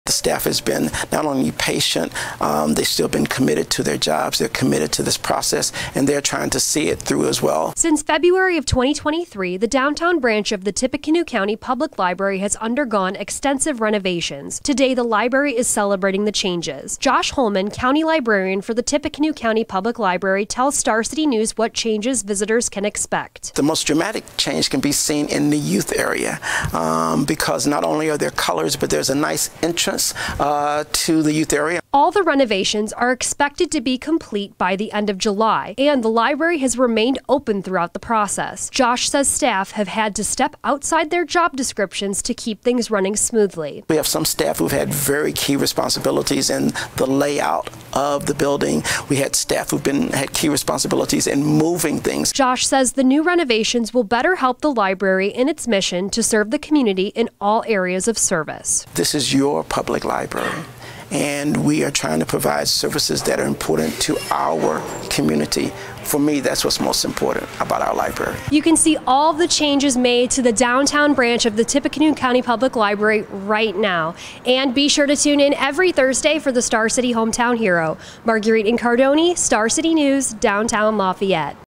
Hometown Heroes: Serving the community without missing a beat
Since February of 2023, the downtown branch of the Tippecanoe County Public Library has undergone extensive renovations. Today the library is celebrating the changes at an open house on Thursday evening.

Star City News got a sneak peek before the event. Jos Holman, County Librarian for the Tippecanoe County Public Library, says library staff have been instrumental throughout the process.